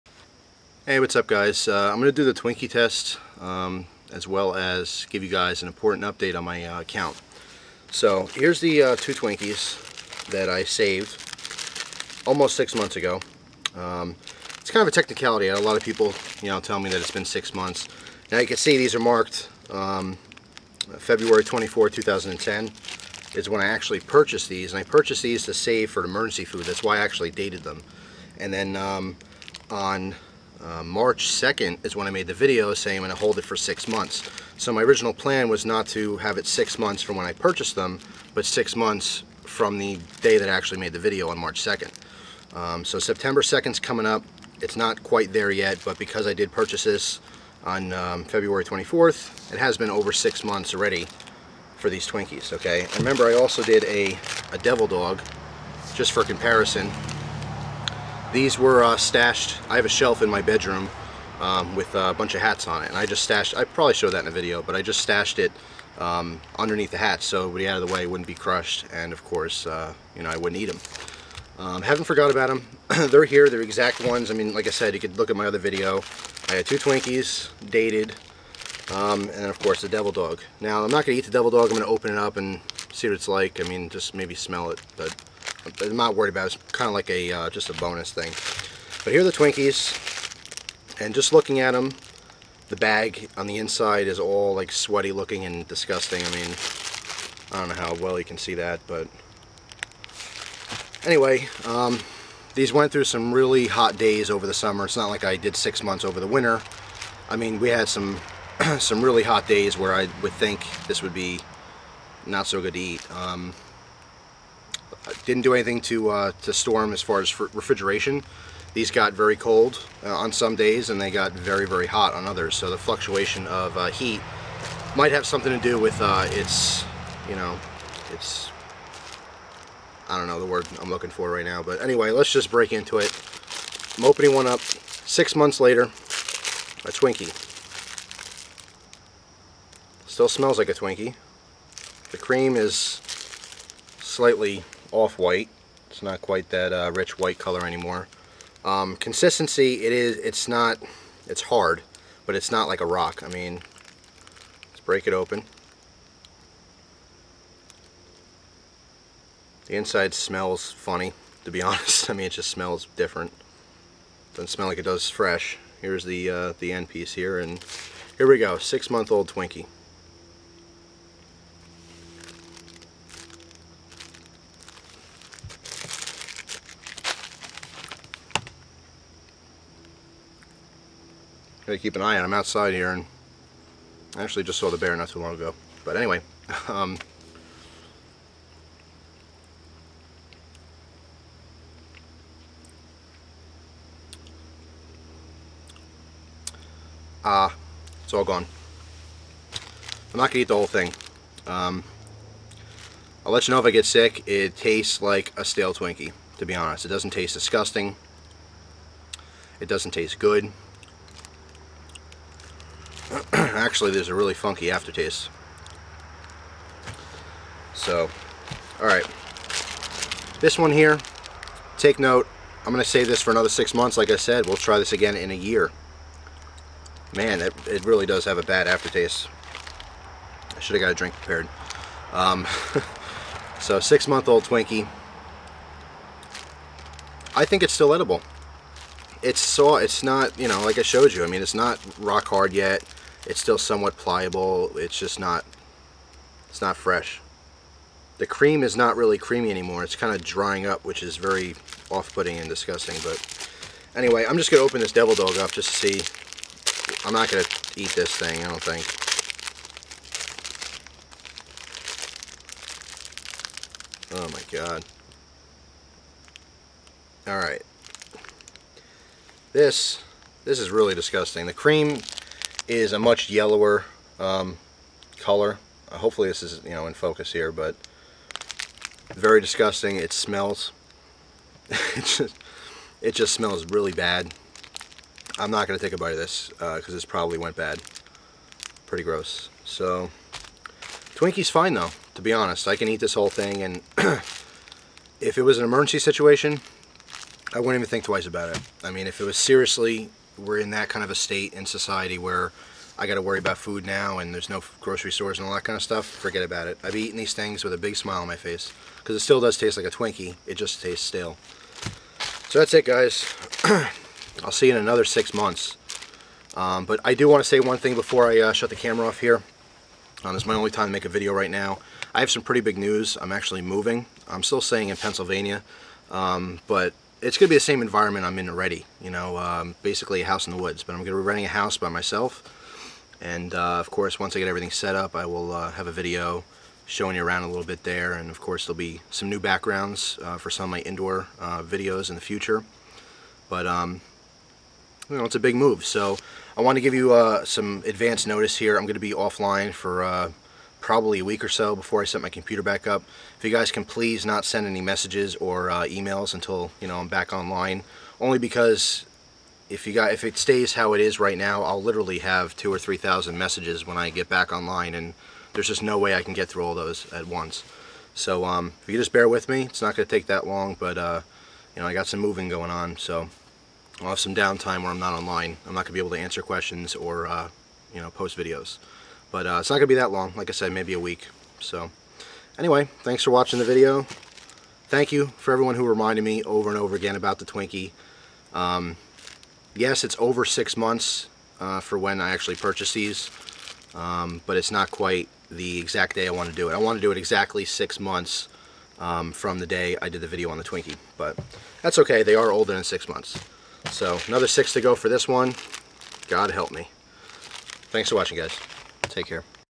Eating 6 Month Old Twinkie + Update/News
Ok guys here it is, 6 months later, time to eat!